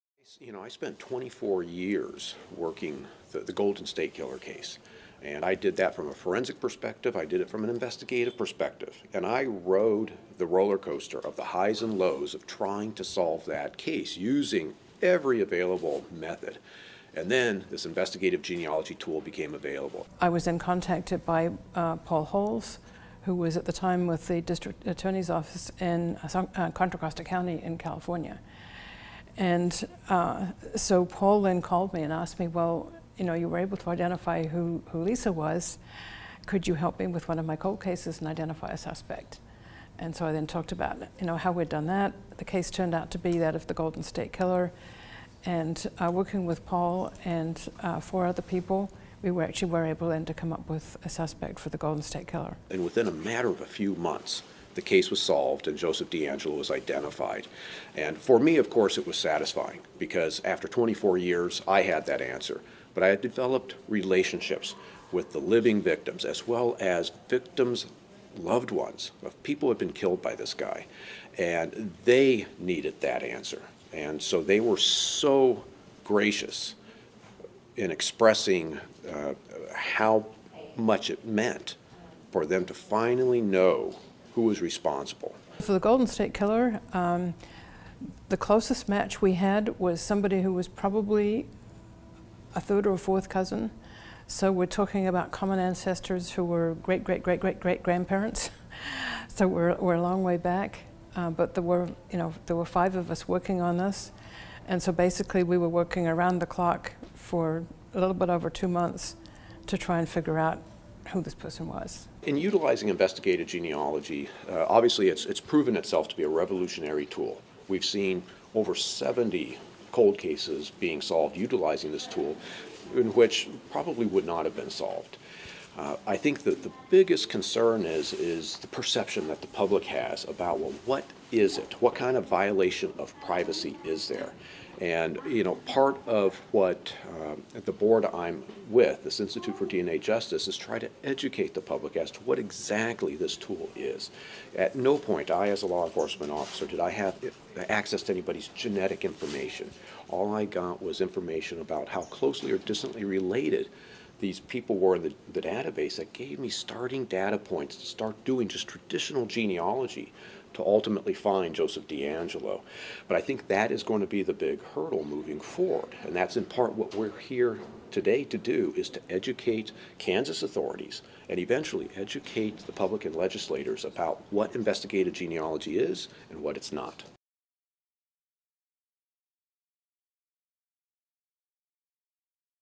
Golden State Killer sleuths share their expertise with Kansas cops
Two of the investigators that identified a suspect in the Golden State Killer case spoke to law enforcement officers in Wichita this week about the emerging field to genetic genealogy and how it can be used to solve cold cases.